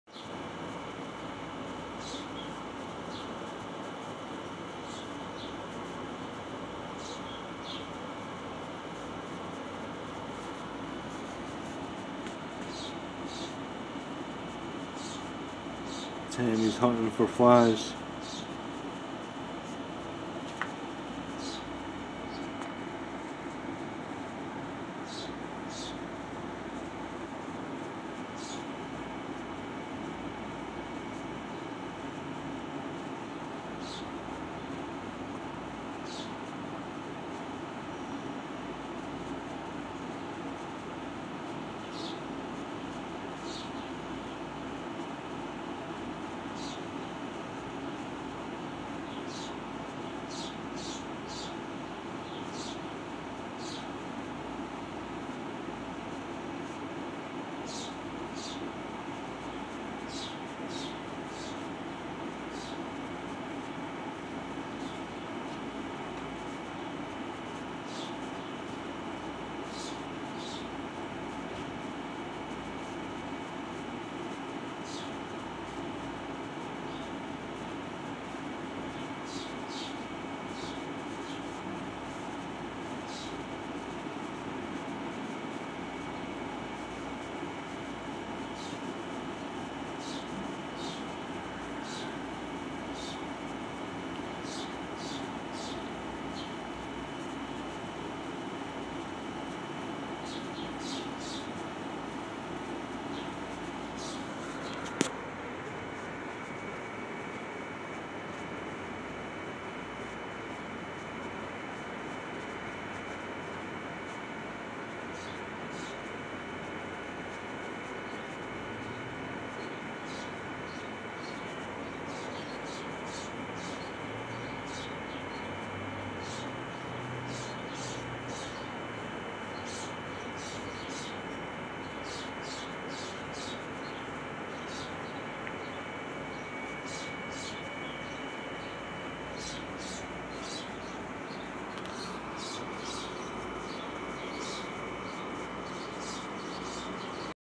Cat hunting flys
Tammy is hunting flys in the yard